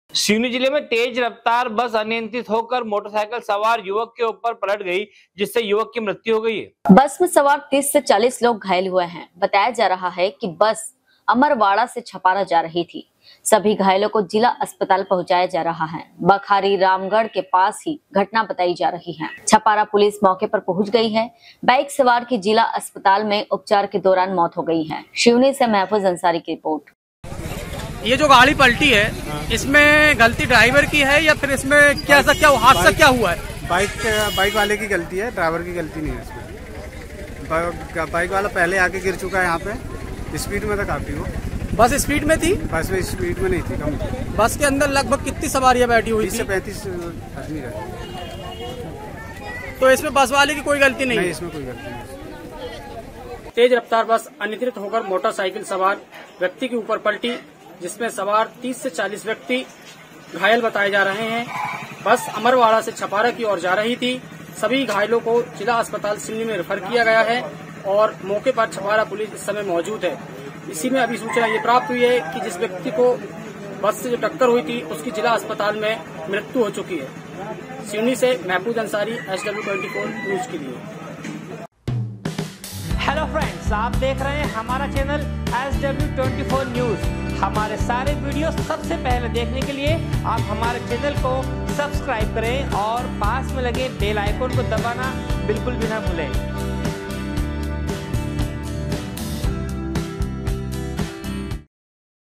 सिवनी जिले में तेज रफ्तार बस अनियंत्रित होकर मोटरसाइकिल सवार युवक के ऊपर पलट गई
सिवनी जिले में तेज रफ्तार बस अनियंत्रित होकर मोटरसाइकिल सवार युवक के ऊपर पलट गई सिवनी से महफूज अंसारी की रिपोर्ट।